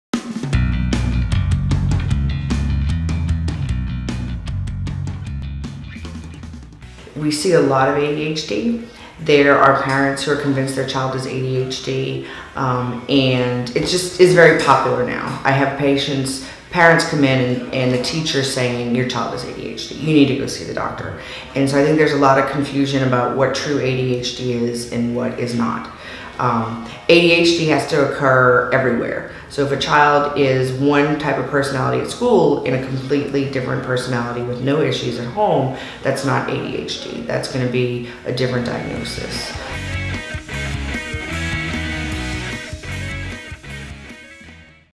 ADHD or Something Else
Here's Dr. Kelly Nation of Gulf Coast Children's Clinic discussing ways you can tell if your child has ADHD or something different.